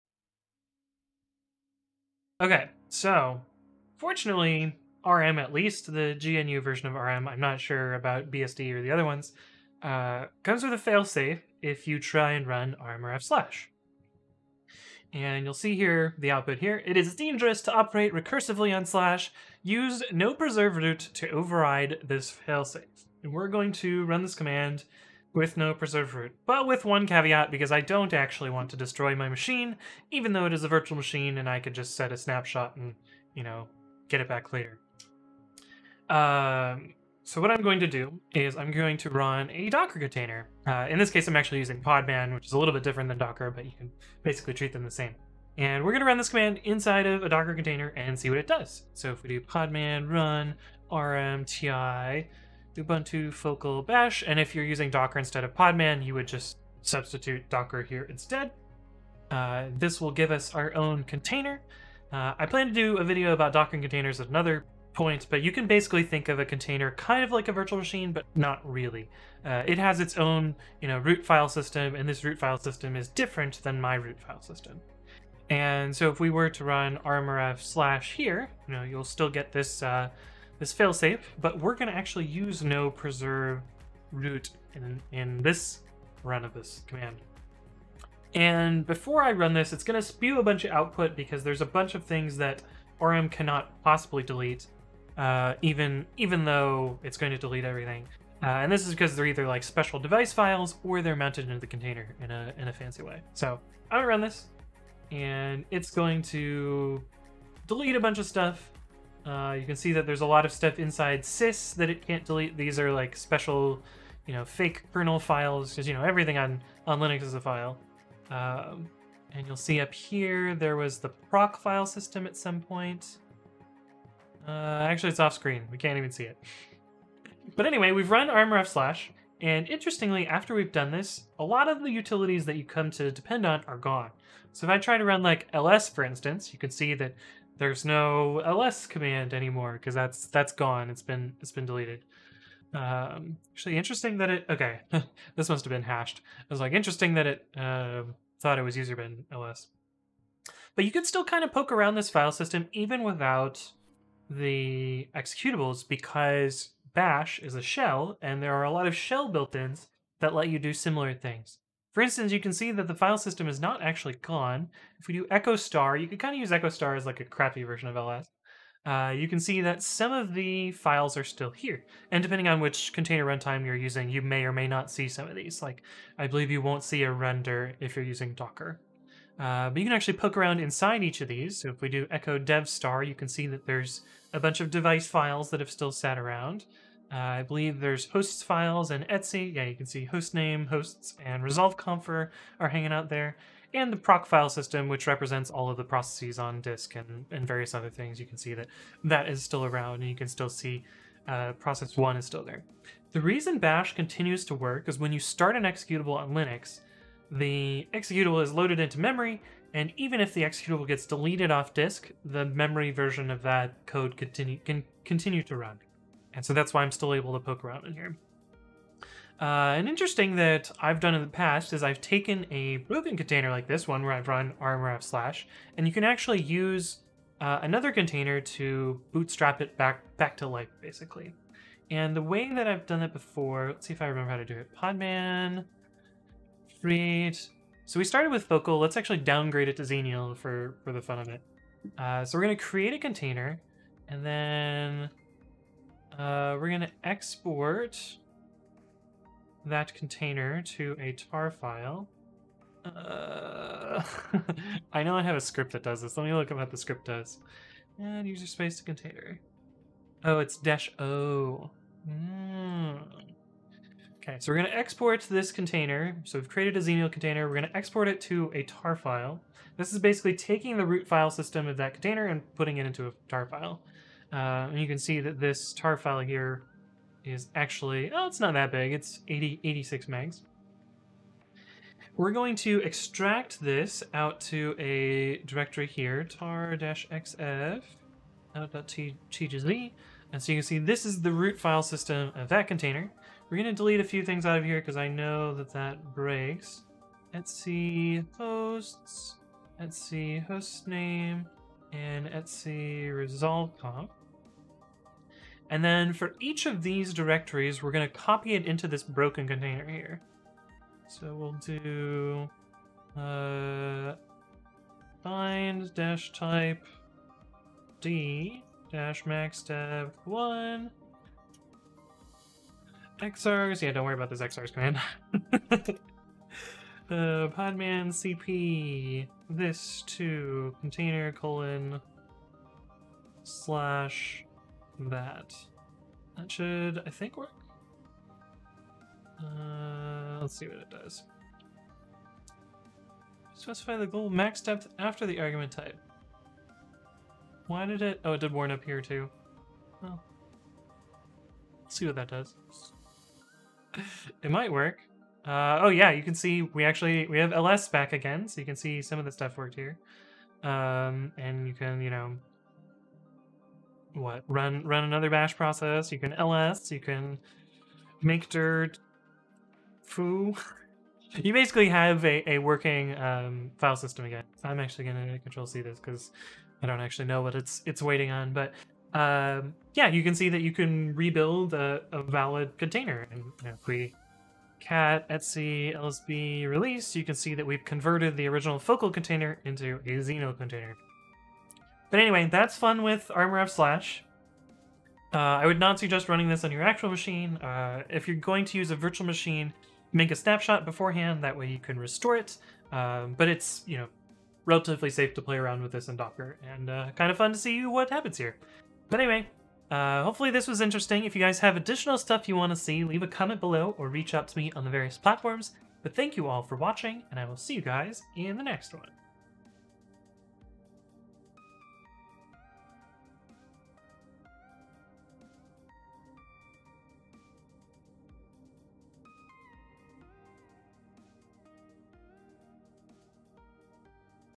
I run 'rm -rf /' and see what happens (beginner - intermediate) anthony explains #091
today I totally destroy my machine by running rm -rf / --no-preserve-root.  let's see what happens!